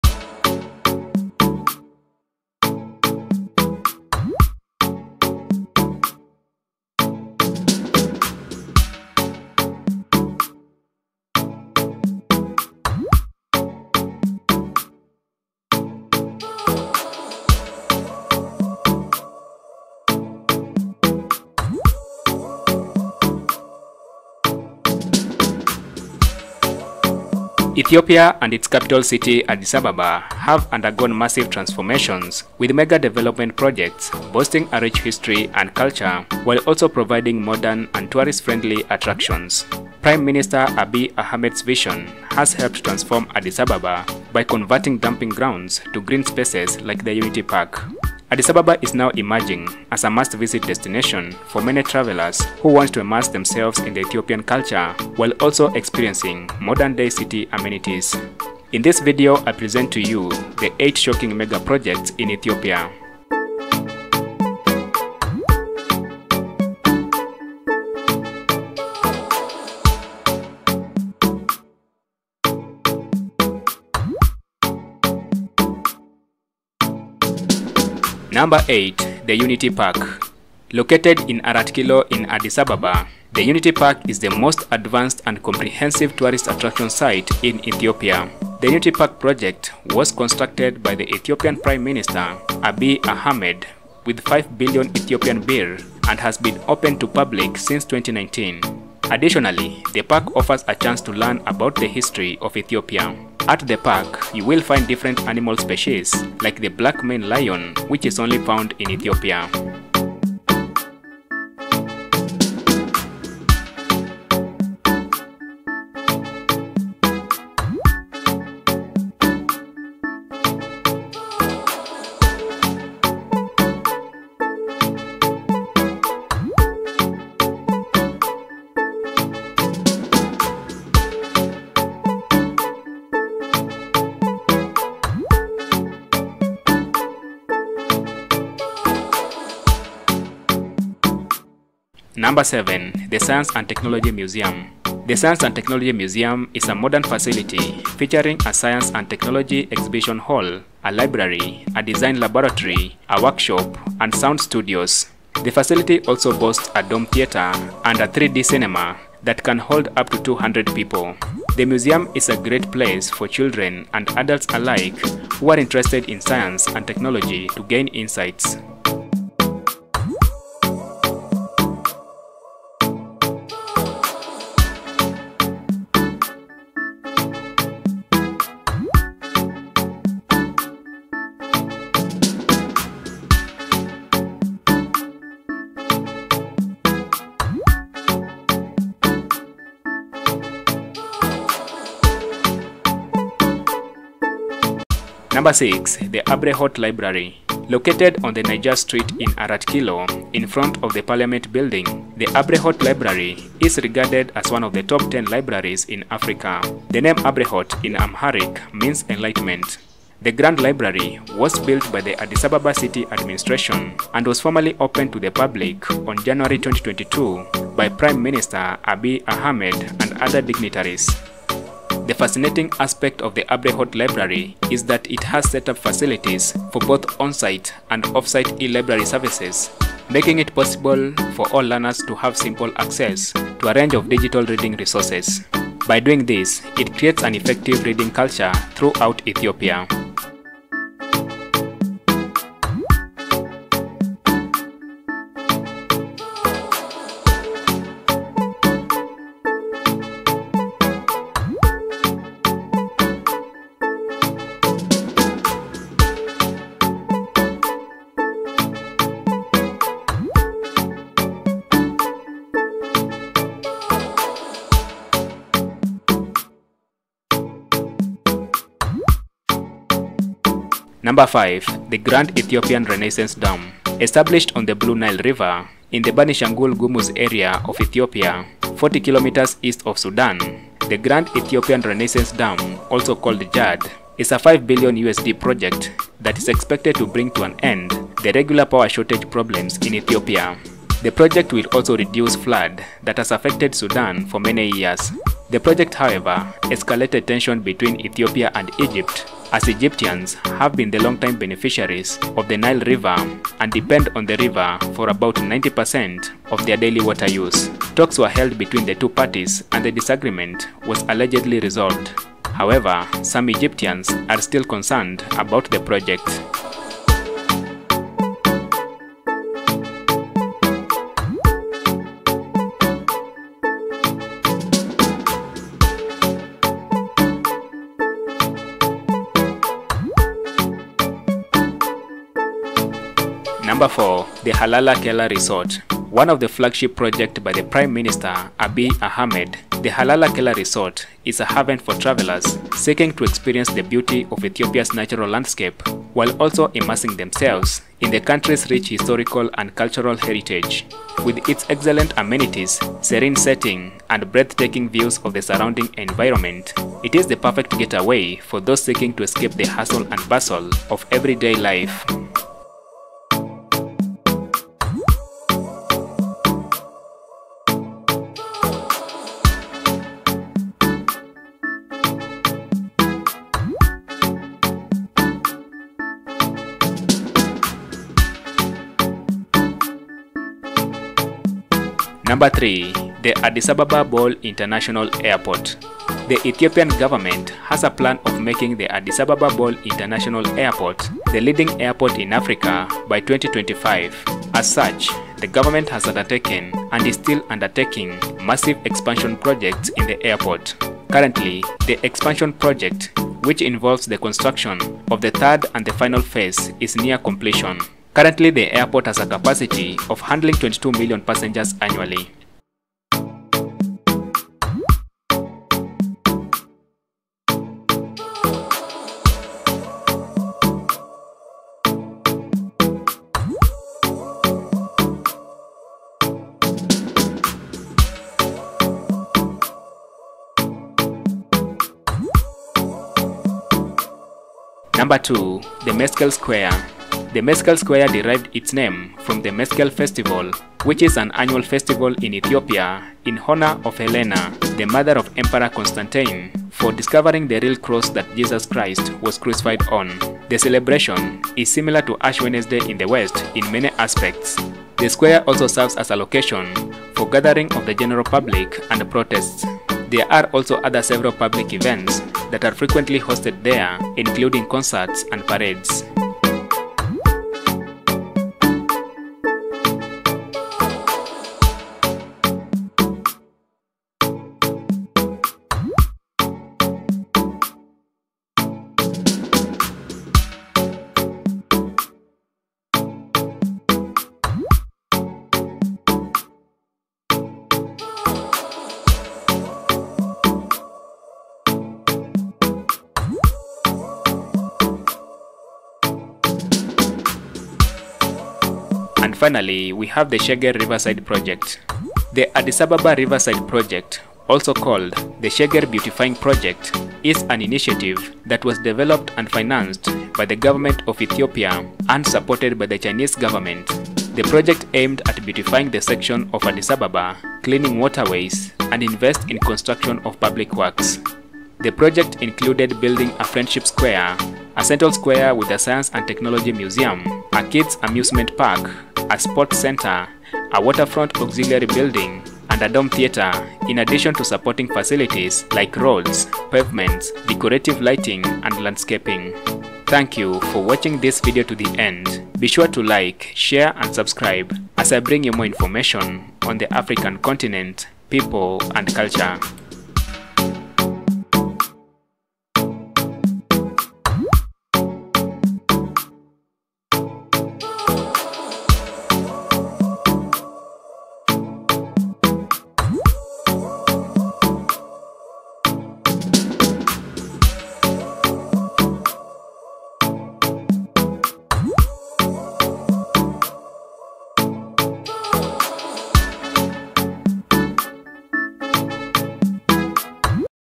8 Completed MEGAPROJECTS in Ethiopia That Have Shocked the World
Both Ethiopia and its Capital City Addis Ababa boast of Megaprojects that have shocked the World. Under the Leadership of Abiy Ahmed, the Prime Minister of Ethiopia, the Country has experienced massive growth in terms of   Infrastructural developments hence boosting the economy of the country.

In this video, I present to you the 8 Completed megaprojects in Ethiopia and its Capital City Addis Ababa.

0:00 - Outro
1:18 - Unity Park Addis Ababa
2:21 - Science and Technology Museum Ethiopia
3:14 - Abrehot Library
4:30 - Grand Ethiopian Renaissance Dam
5:33 - Halala Kela Resort
6:21 - Addis Ababa Bole International Airport
7:14 - Meskel Square
8:19 - Sheger Riverside Project
9:00 - Outro